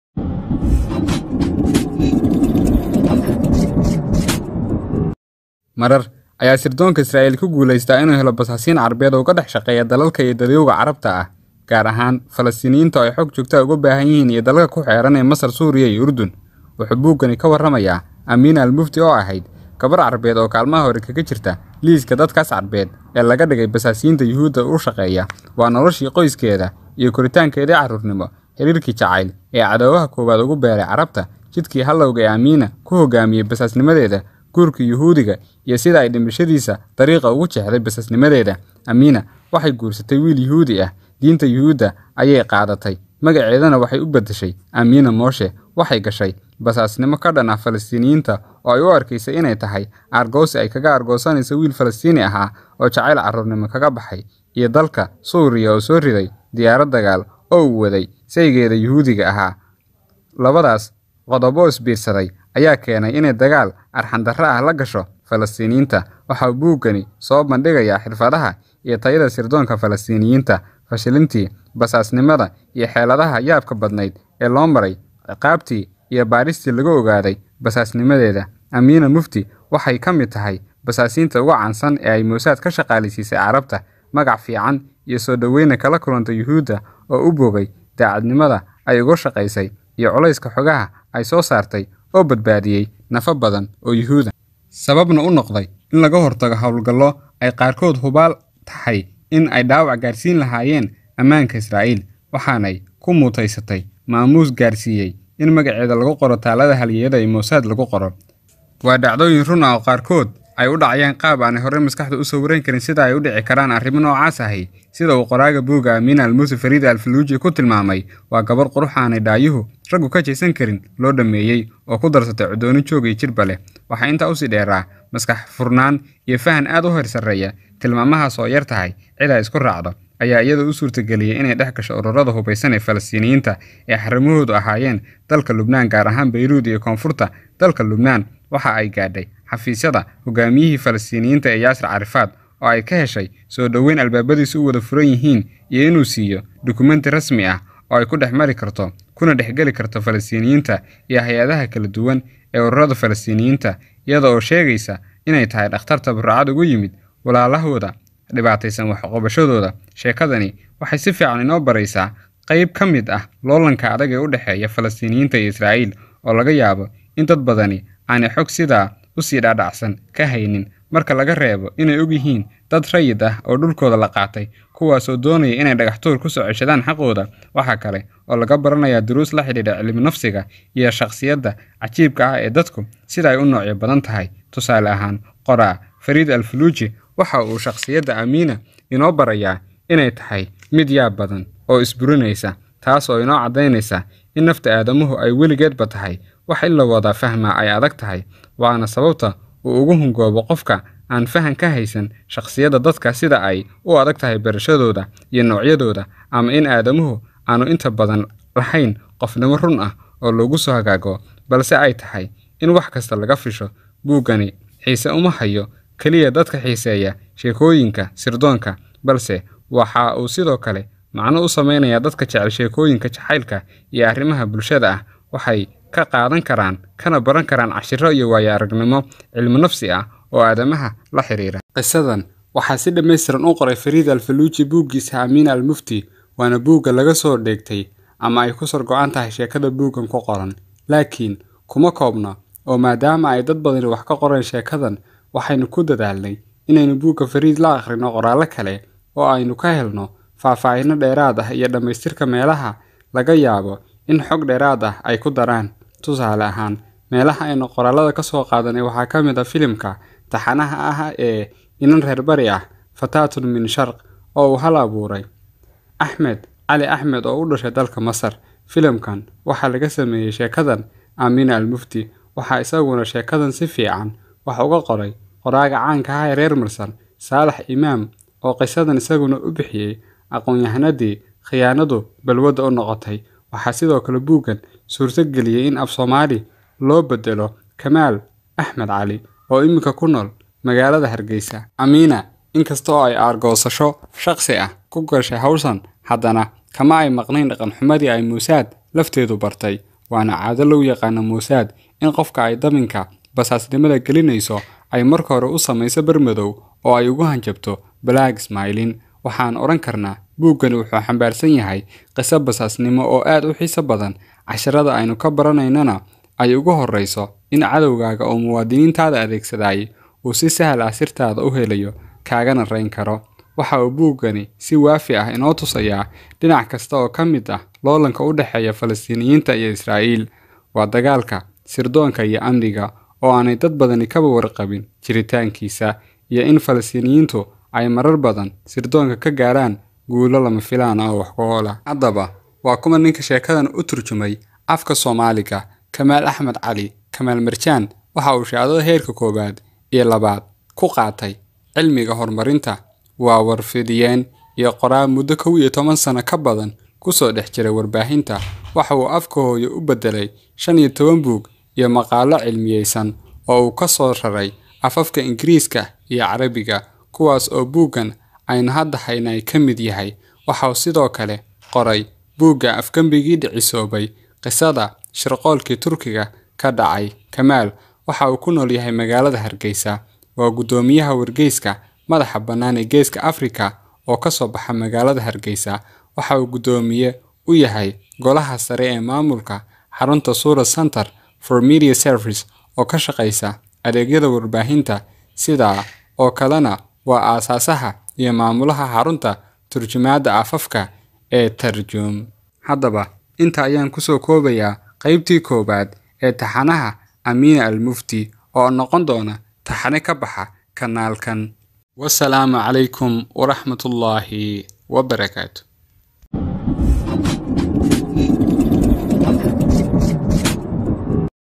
Amina almuft 1
Basaasadii carbeed ee mossad u shaqayn jirtay